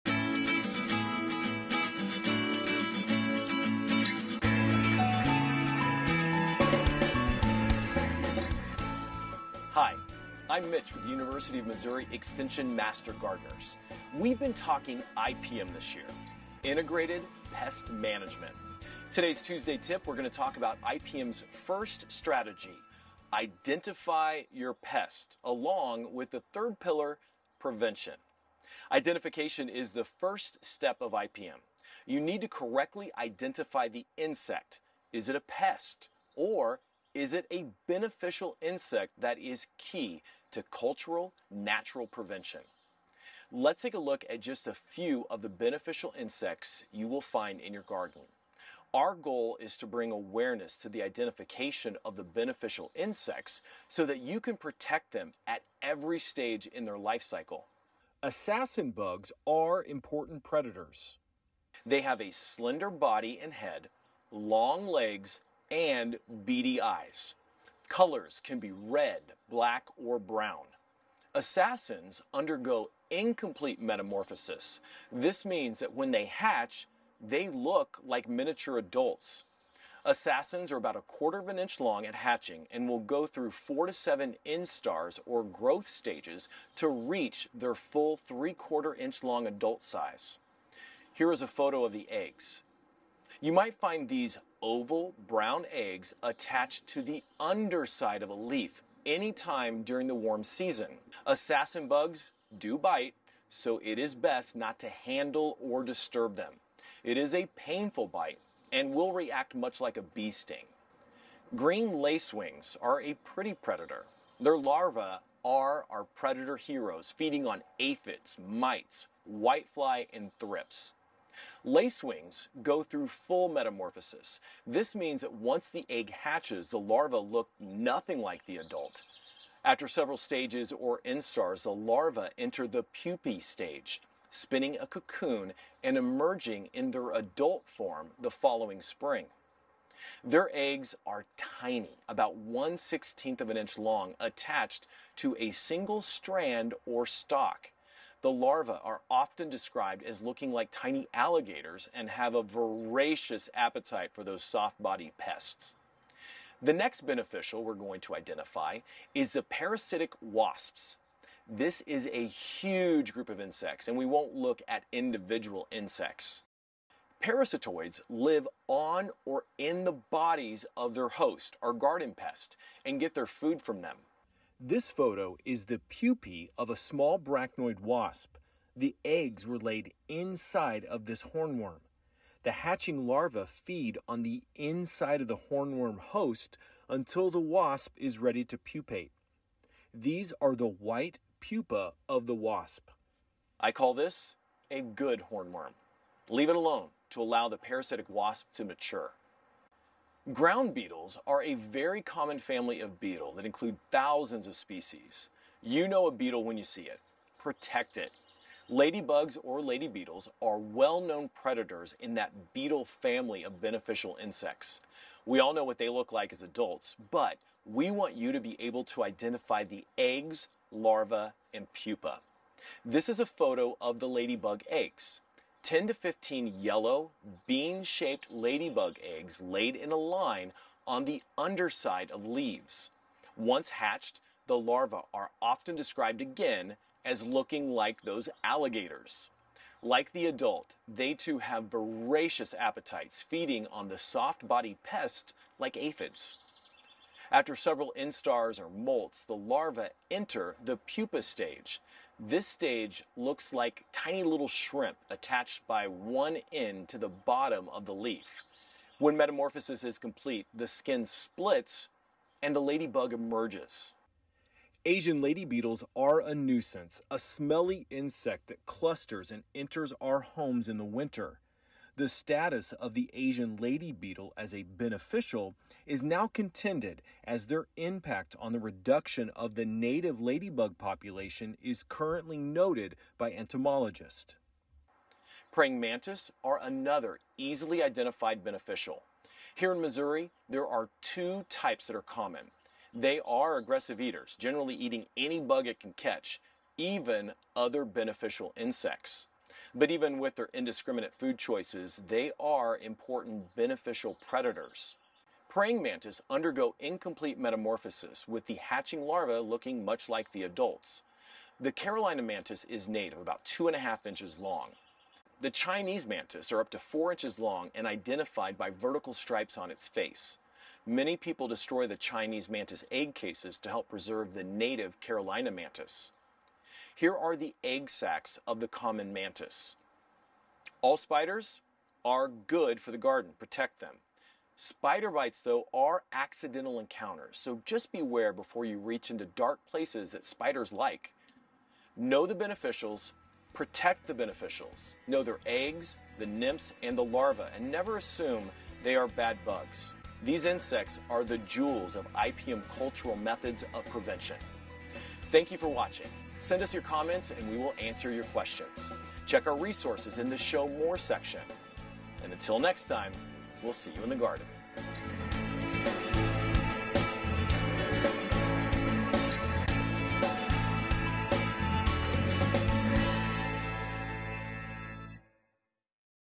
IPM - Know the Beneficials
The first pillar of Integrated Pest Management (IPM) is to IDENTIFY your pest!  Know which insect is a pest, and which ones are beneficial.  Beneficial insects are the jewels of cultural methods of the third pillar of IPM, PREVENTION.

This Tuesday Tip will introduce you to common beneficial insects and other arthopods, from eggs - to - adult.
Know the beneficials, and protect the beneficials.
Never assume that insect you found is a  bad bug!

Resources:
Our favorite Garden Journal from MU!

Consider visiting your local extension office for more research-based resources.

Garden Questions?

Credits:
Produced by  - Tuesday Tip Team
Dr. Tamra Reall, Field Specialist in Horticulture, University of Missouri Extension
Written by - Sherri Carter
Edited by - Mitch Weber

Photo Credits:
North American Wheel Bug nymphs
Lacewing cocoon - Celeste Welty, Ohio State University, Bugwood.org, creativecommons.org Attribution-Noncommercial 3.0 License
Lacewing eggs - Ansel Oommen, Bugwood.org,
Chinese Mantis - Gerald Holmes, Strawberry Center, Cal Poly San Luis Obispo, Bugwood.org,
all other photos, Cyberlink subscription with Getty images

The University of Missouri is an equal opportunity/access/affirmative action/pro-disabled and veteran employer.